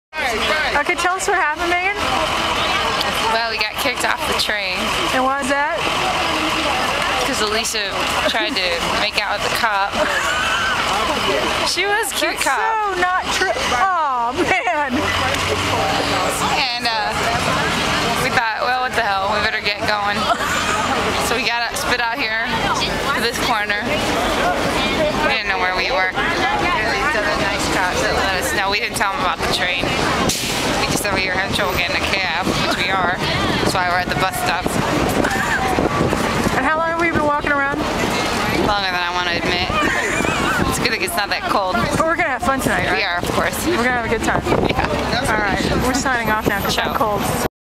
Kicked off the F Train in Brooklyn
Megan explaining what happened to us on the train.  Actually there was an accident and we got booted off the train, but Meg makes up her own explanation